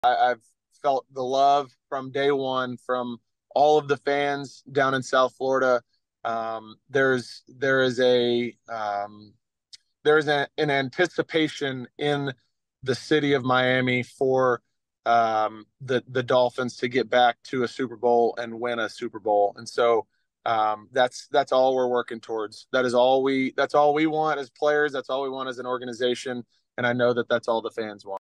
Miami Dolphins Long Snapper Blake Ferguson
Meet Miami Dolphins Long Snapper as he shares his story of going on a once in a lifetime trip to Fort Jackson where he got to see first hand how the Army Basic training is conducted.